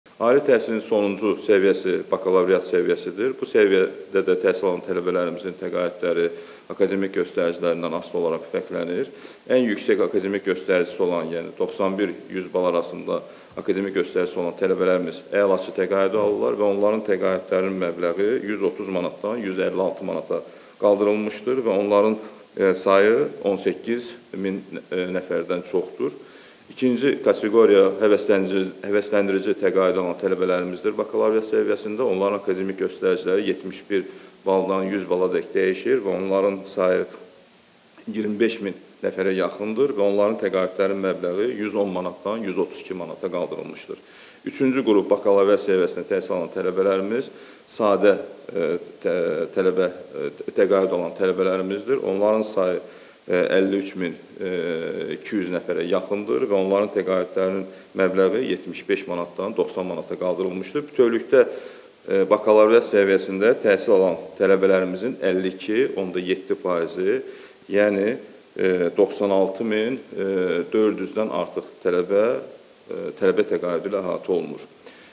Tələbələrin təqaüdü nə qədər artıb?- Nazir müavinindən açıqlama
Prezident İlham Əliyev “Doktorantlara, ali təhsil, orta ixtisas və peşə təhsili müəssisələrində, həmçinin Azərbaycan Milli Elmlər Akademiyasının magistratura səviyyəsində təhsil alan tələbələrə təqaüdlərin verilməsi haqqında” Azərbaycan Respublikası Prezidentinin 2019-cu il 14 fevral tarixli 961 nömrəli Sərəncamında dəyişiklik edilməsi barədə Sərəncam imzalayıb.
bakalavriat səviyyəsi üzrə:
akademik göstəriciləri 91-100 bal olan tələbələrə verilən təqaüdün məbləği 130 manatdan 156 manatdək,
akademik göstəriciləri 71-100 bal olan tələbələrə verilən təqaüdün məbləği 110 manatdan 132 manatadək,
akademik göstəriciləri 51-100 bal olan tələbələrə verilən təqaüdün məbləği 75 manatdan 90 manatadək artırılıb.